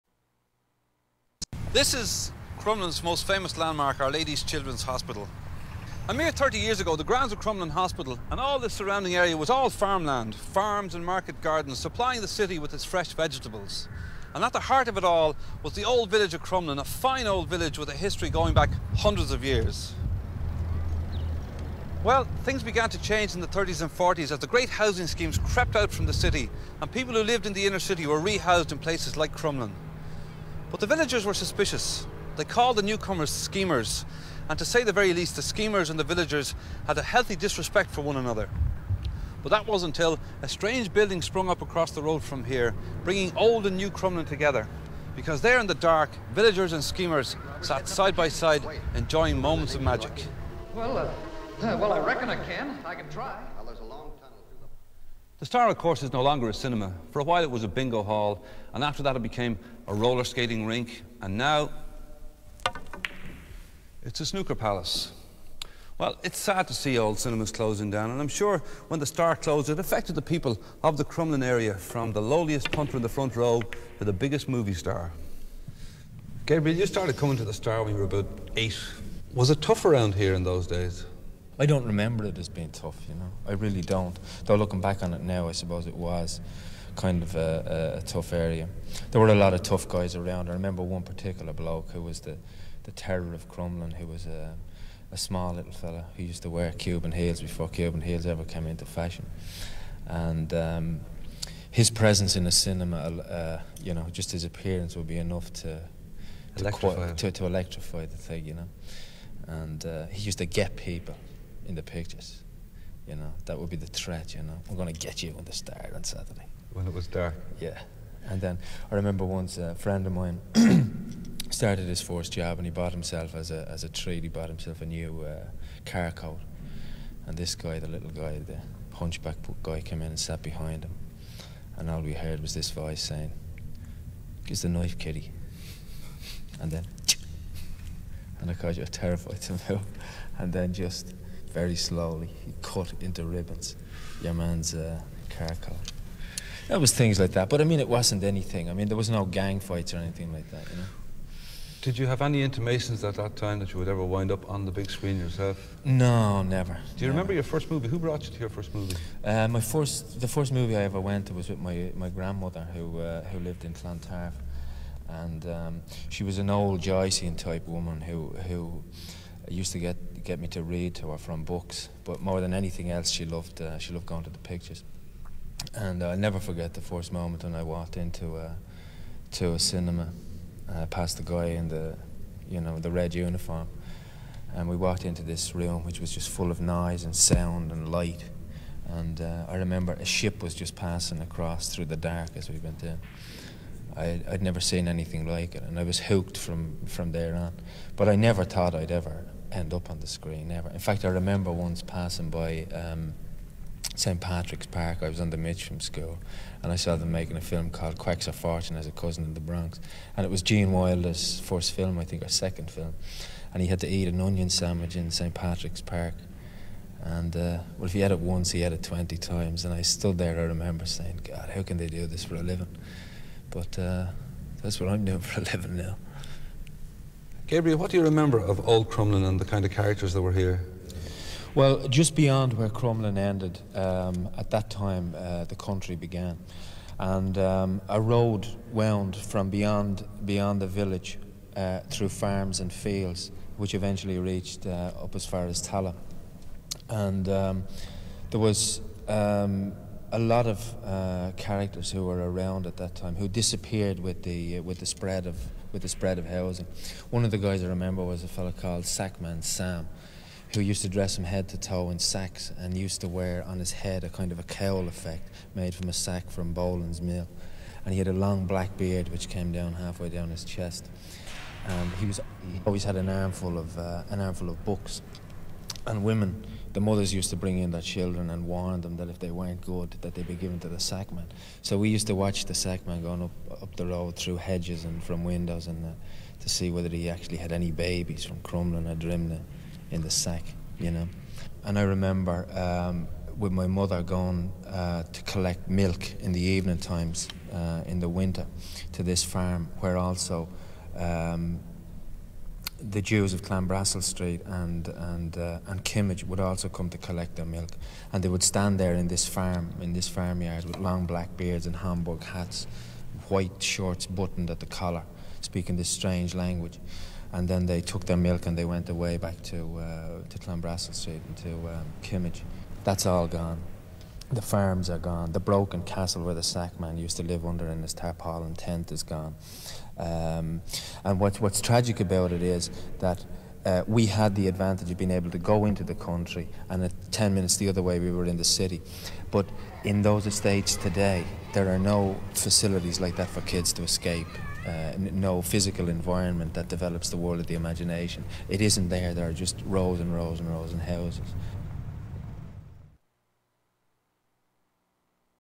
CRUMLIN: THE STAR CINEMA WITH GABRIEL BYRNE 1988
Shay Healy looks at the history of Crumlin's Star Cinema and the impact on the area. Actor Gabriel Byrne reminisces about growing up in the area, from his birth in nearby Drimnagh to moving to Walkinstown just as he started school.